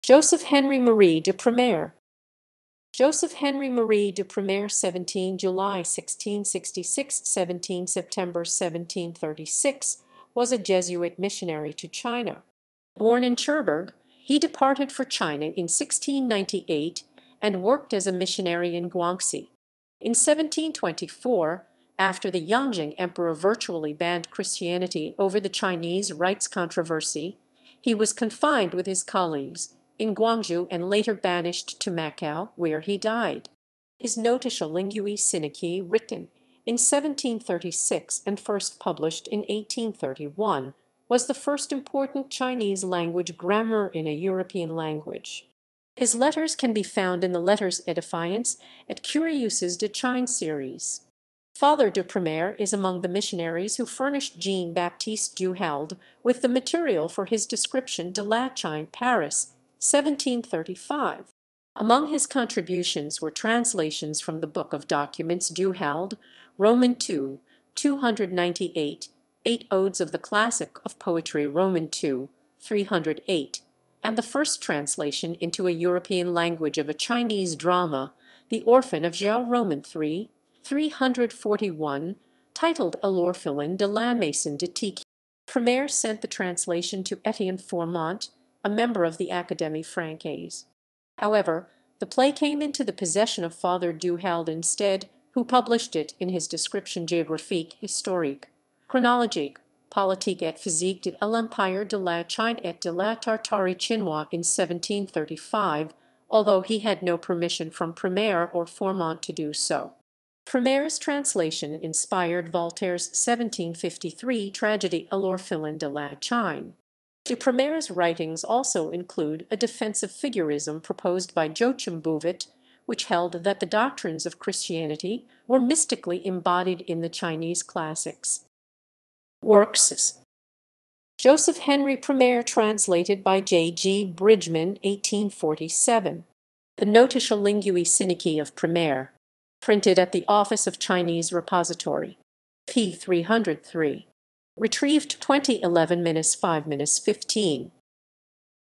Joseph Henri Marie de Prémare
Joseph Henri Marie de Prémare (17 July 1666 – 17 September 1736) was a Jesuit missionary to China. Born in Cherbourg, he departed for China in 1698, and worked as a missionary in Guangxi.
In 1724, after the Yongzheng Emperor virtually banned Christianity over the Chinese Rites controversy, he was confined with his colleagues in Guangzhou and later banished to Macau, where he died.
His Notitia linguae sinicae, written in 1736 and first published in 1831, was the first important Chinese language grammar in a European language. His letters can be found in the Lettres édifiantes et curieuses de Chine series.Father de Prémare is among the missionaries who furnished Jean-Baptiste Du Halde with the material for his "Description de la Chine" (Paris, 1735). Among his contributions were translations from the Book of Documents (Du Halde, II, 298); eight odes of the Classic of Poetry (II, 308); and the first translation into a European language of a Chinese drama, "The Orphan of Zhao" (III, 341), titled L'Orphelin de la Maison de Tchao. Premaré sent the translation to Étienne Fourmont, a member of the Académie française. However, the play came into the possession of Father Du Halde instead, who published it in his Description Géographique, Historique, Chronologique, Politique et Physique de l'Empire de la Chine et de la Tartarie Chinois in 1735, although he had no permission from Prémare or Fourmont to do so. Prémare's translation inspired Voltaire's 1753 tragedy L'Orphelin de la Chine.De Prémare's writings also include a defense of figurism proposed by Joachim Bouvet, which held that the doctrines of Christianity were mystically embodied in the Chinese classics.